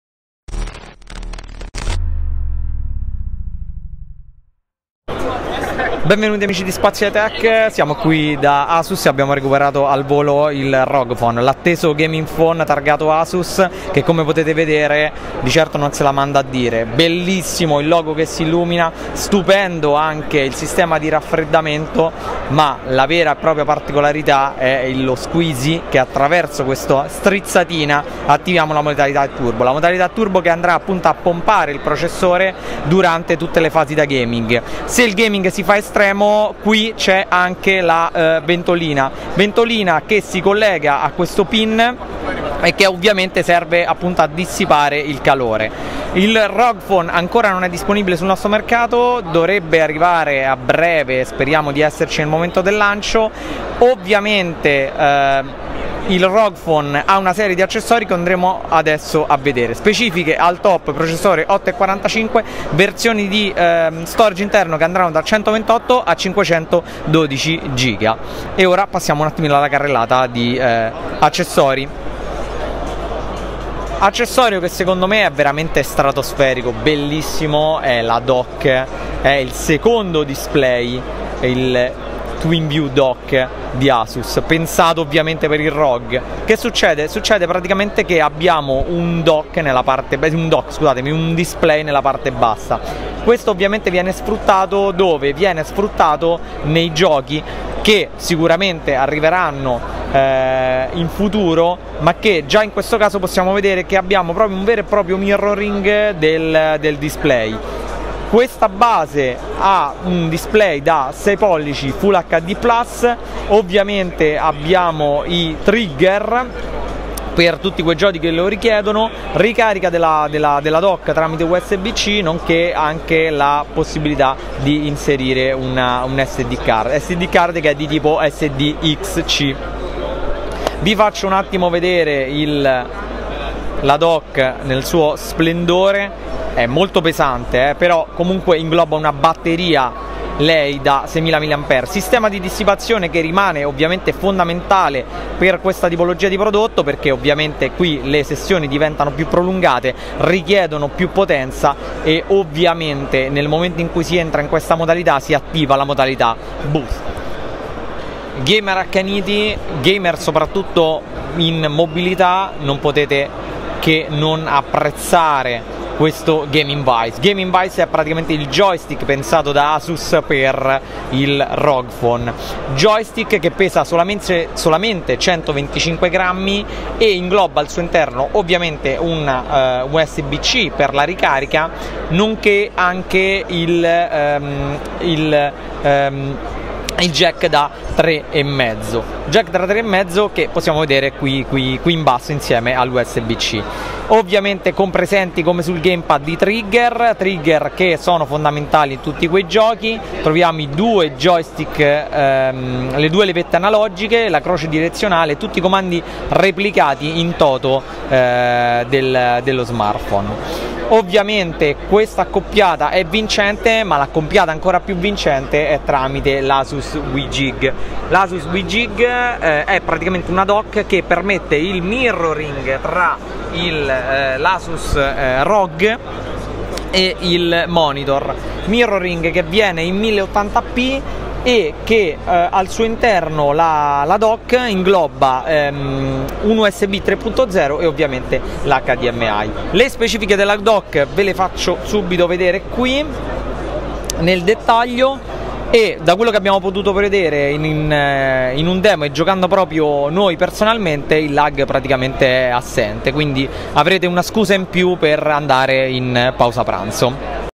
ASUS ROG Phone - FOCUS IFA2018 di Spazio iTech
IFA 2018 - ASUS ROG PHONE, il primo smartphone da GAMING by Asus!
Ecco le nostre prime impressioni!
Se vi fosse piaciuto il video vi invitiamo a mettere un "mi piace". Ogni critica costruttiva è ben accetta!